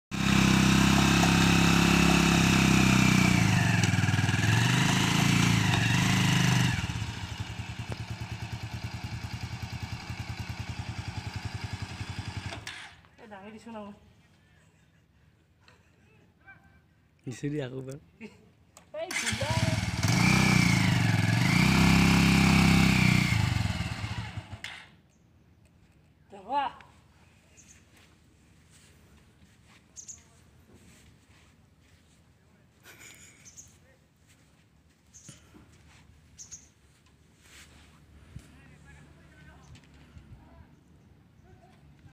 When my bike sink in sand
bike got stuck in sand, just like bike stunt,